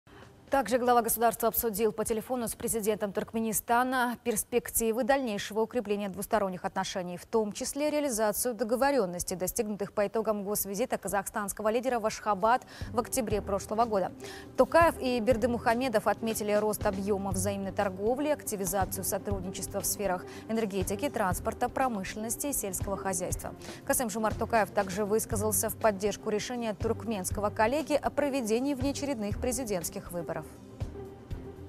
Состоялся телефонный разговор Президента РК с главой Туркменистана
К. Токаев обсудил по телефону с президентом Туркменистана перспективы дальнейшего укрепления двусторонних отношений

В том числе реализацию договоренностей, достигнутых по итогам госвизита казахстанского лидера в Ашхабад в октябре прошлого года.

Токаев и Бердымухамедов отметили рост объемов взаимной торговли, активизацию сотрудничества в сферах энергетики, транспорта, промышленности и сельского хозяйства. Касым-Жомарт Токаев также высказался в поддержку решения туркменского коллеги о проведении внеочередных президентских выборов.